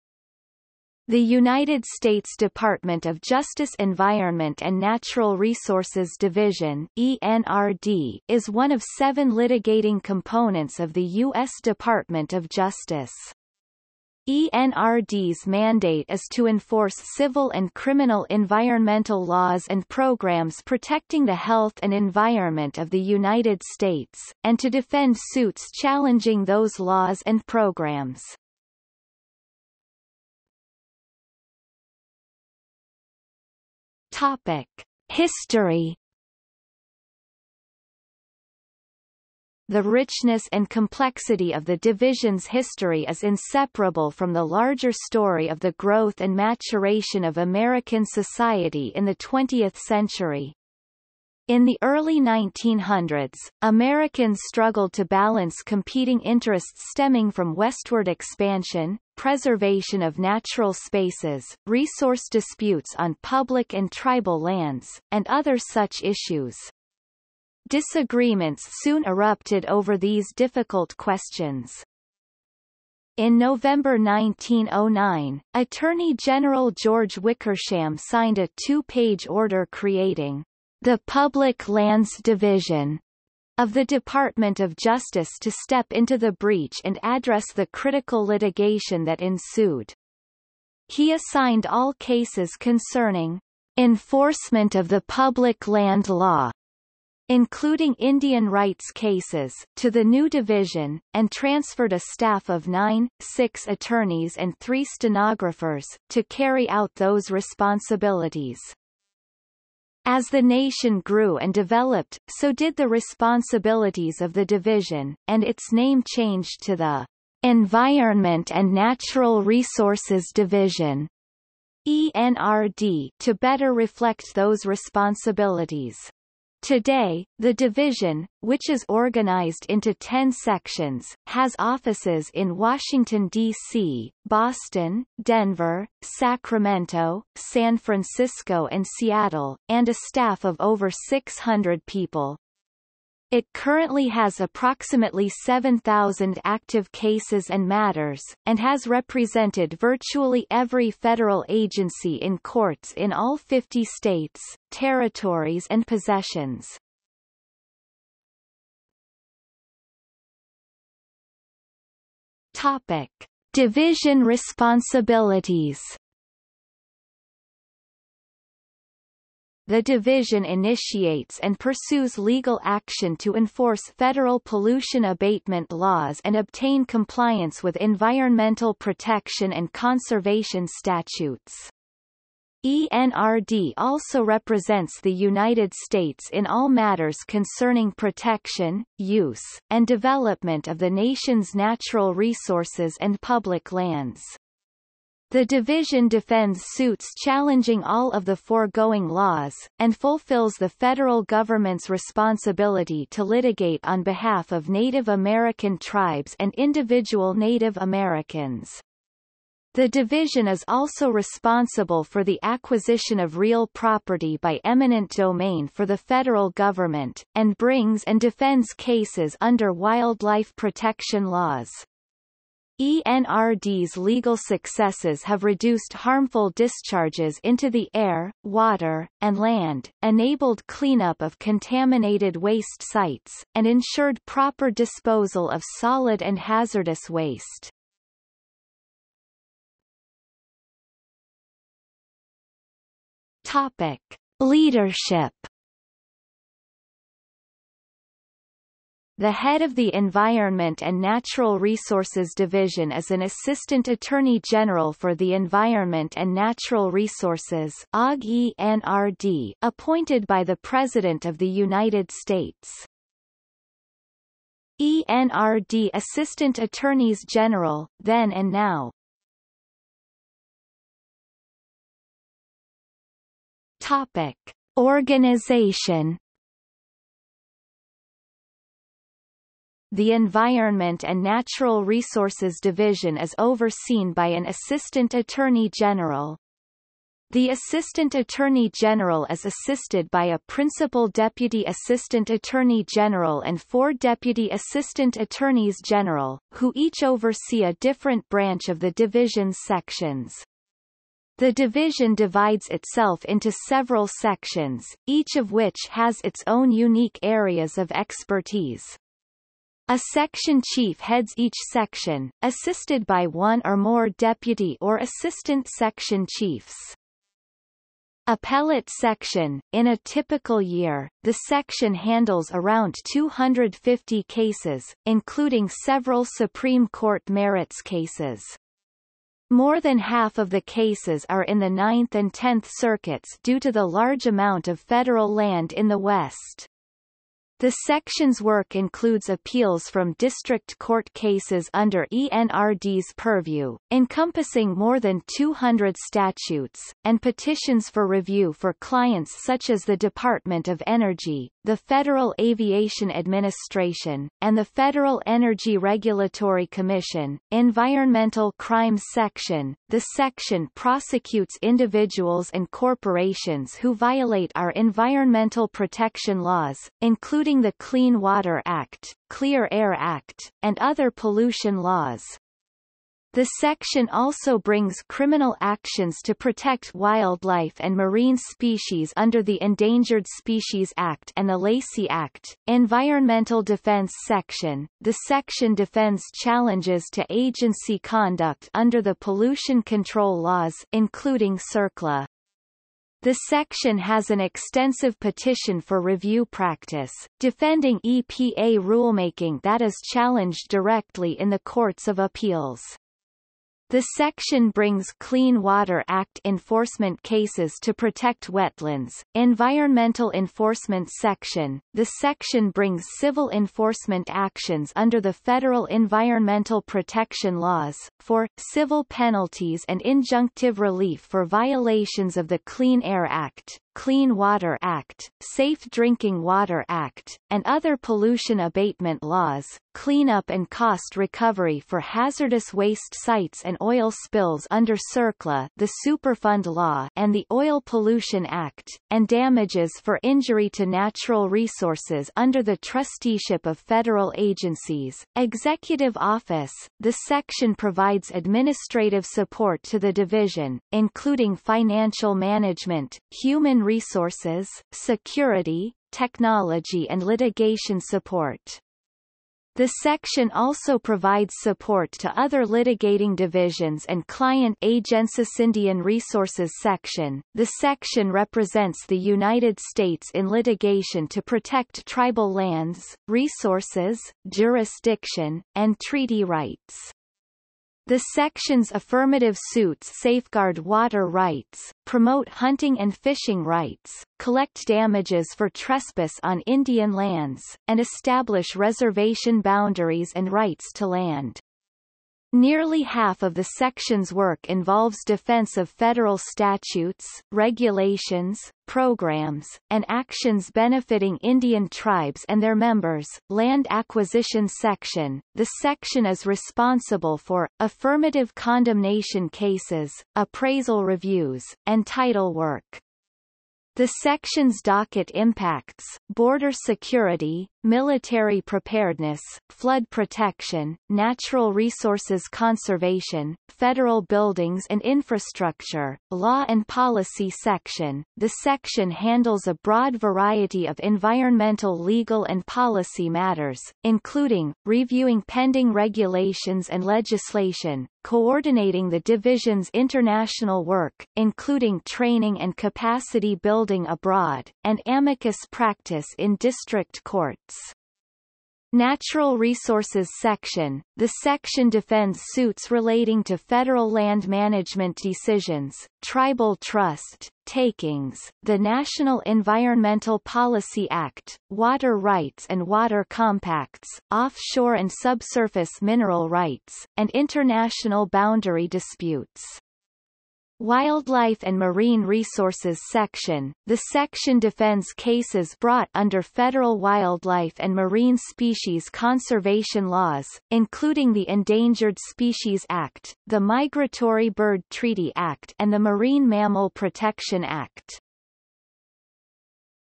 United States Department of Justice Environment and Natural Resources Division | Wikipedia audio ...
00:00:30 1 History
00:02:38 2 Division responsibilities
00:03:59 3 Leadership
00:04:30 4 Organization

- Socrates

SUMMARY
The United States Department of Justice Environment and Natural Resources Division (ENRD) is one of seven litigating components of the U.S. Department of Justice. ENRD's mandate is to enforce civil and criminal environmental laws and programs protecting the health and environment of the United States, and to defend suits challenging those laws and programs.